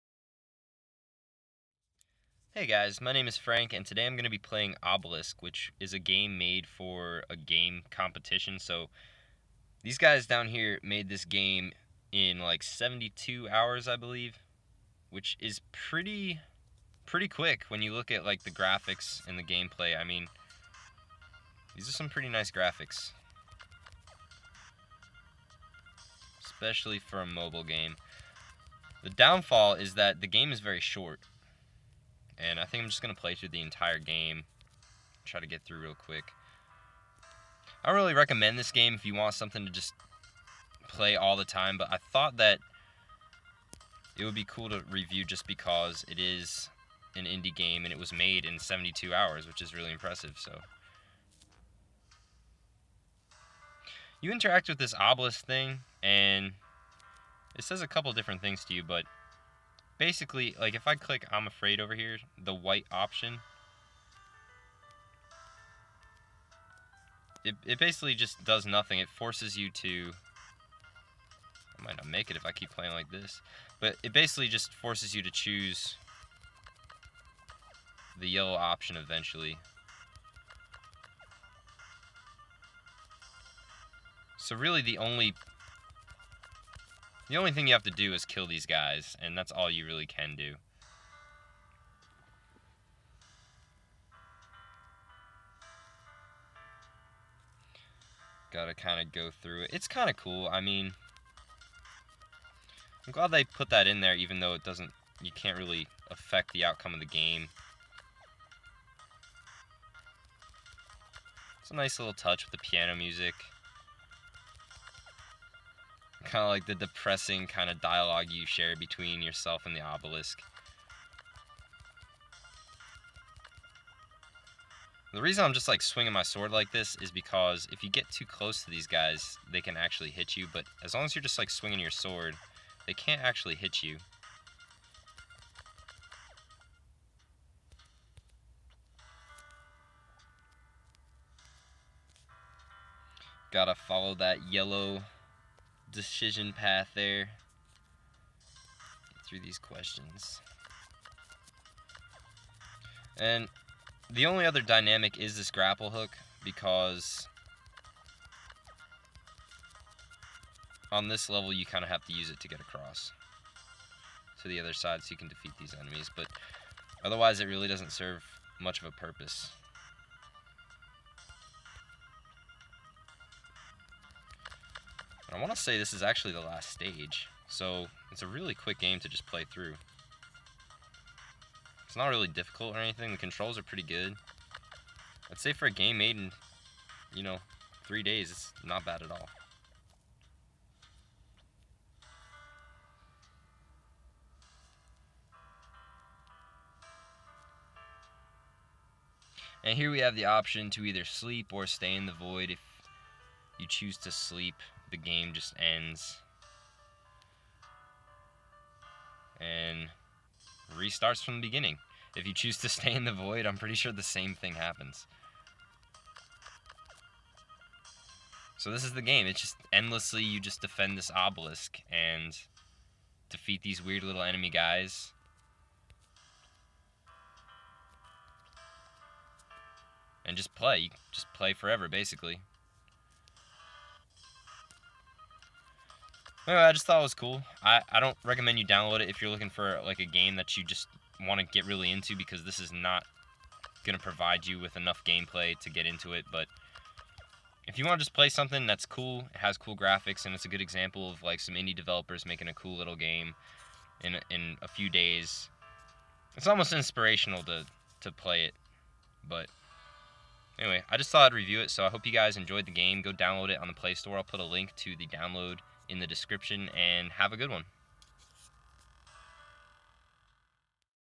Obelisk - Short Game Review!
This is Obelisk, a hack and slash game created by Team Terrible in less than 72 hours for a Ludum Dare game design competition. It's short, but pretty cool. So give it a playthrough!

I couldn't find much on Team Terrible, but here's a link to the Ludum Dare forum on Obelisk